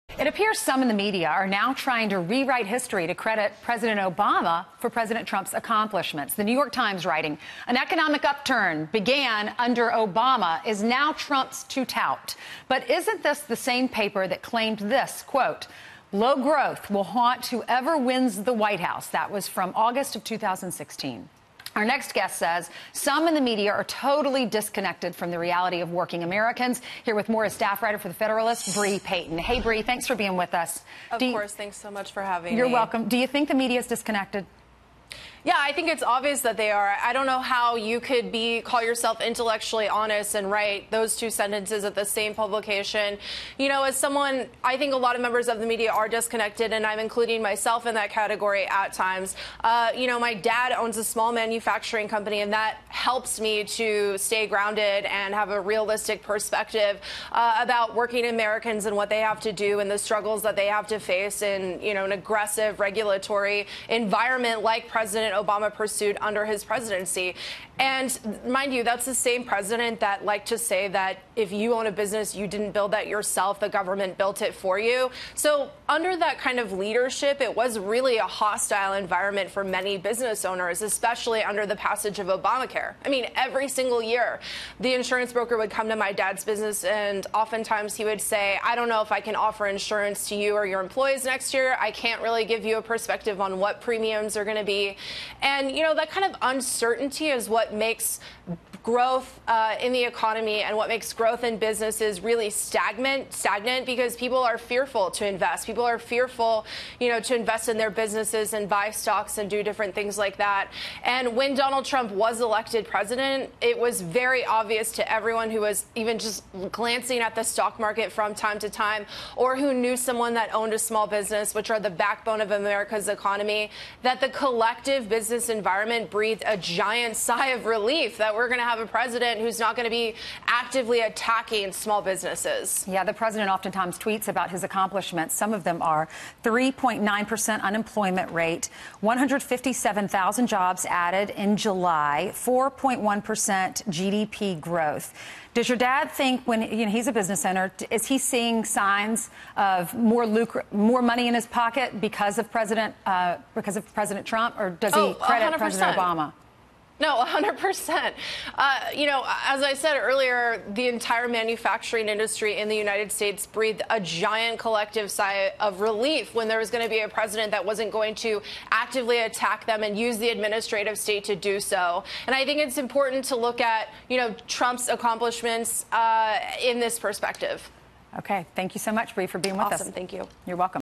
Media rewriting history to credit Obama for Trump’s wins Fox News
New York Times says President Trump is touting Obama's economic growth after writing in 2016 that low growth will haunt whoever wins the White House.